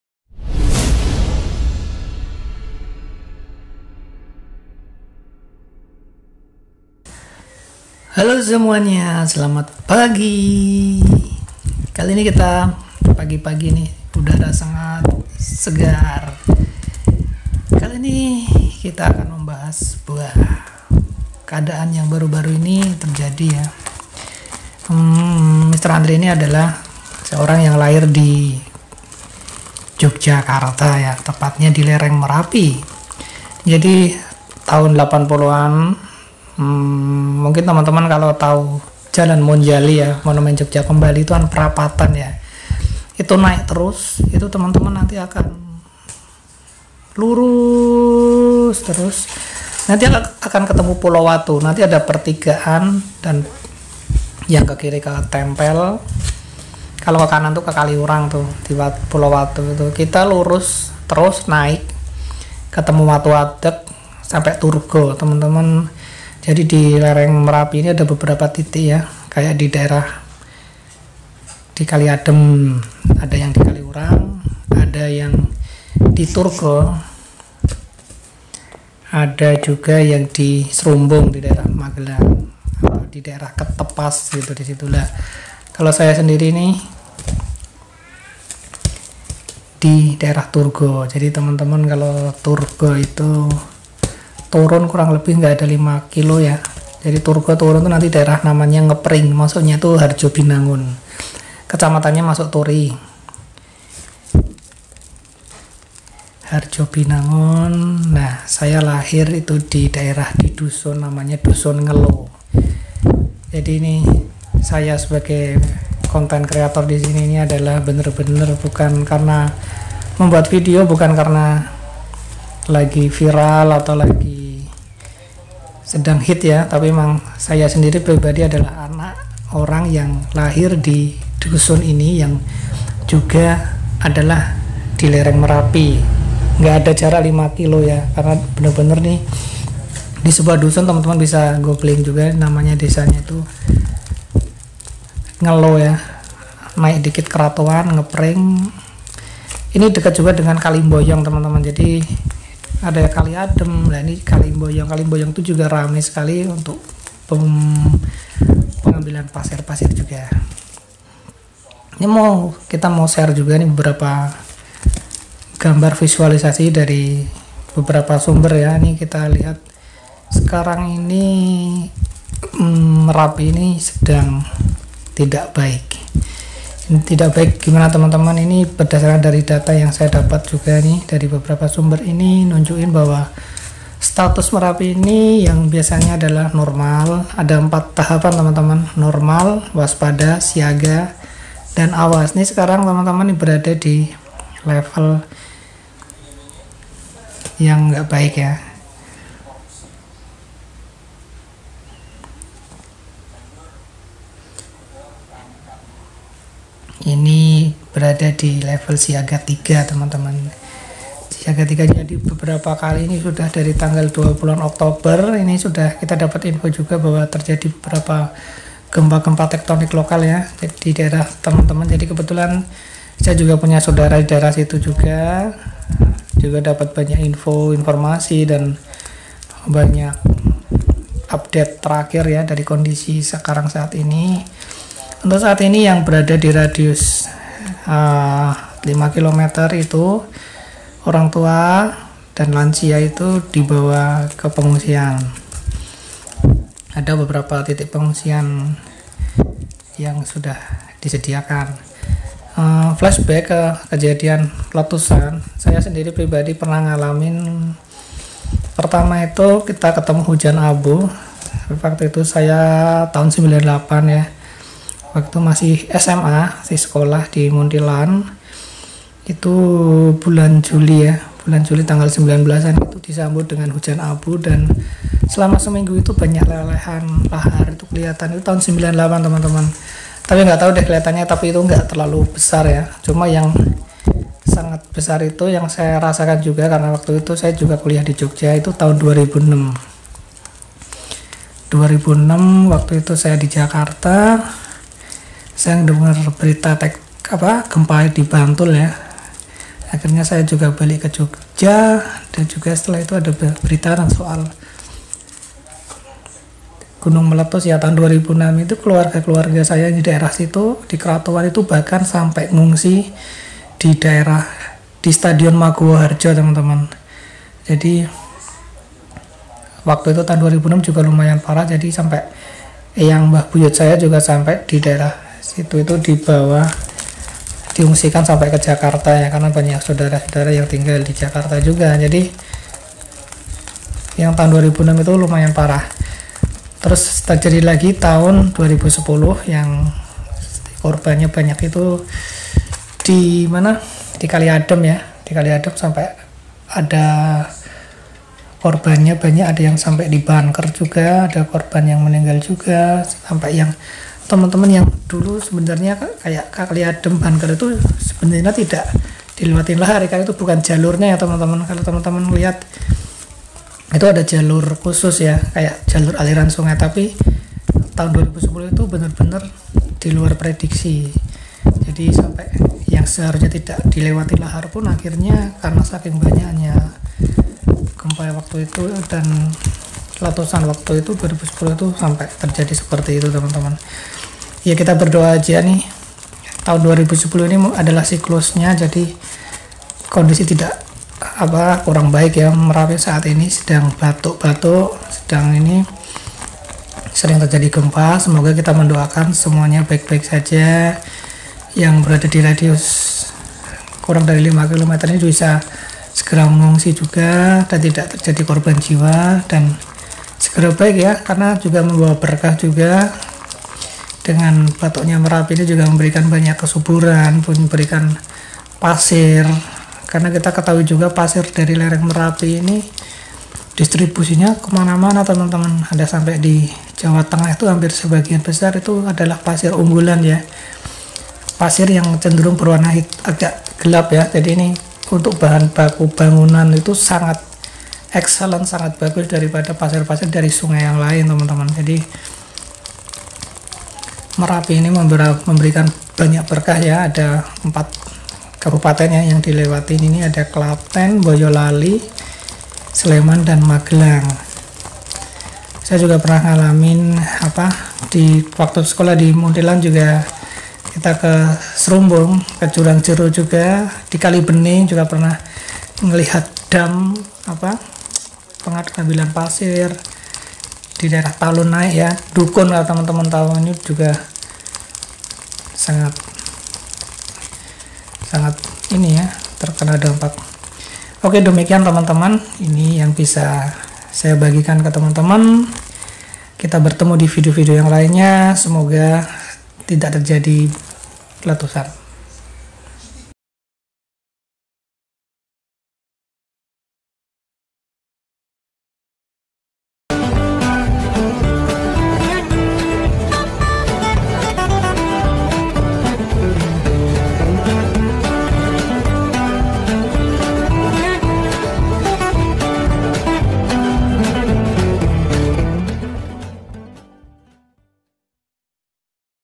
KONDISI MERAPI TERKINI 2020 DAN PENGALAMAN BERSAMA MERAPI - TENG TENG CRIT 01 - TENGUK TENGUK CRITO
Kondisi merapi November 2020
TENG TENG CRIT
TENGUK TENGUK CRITO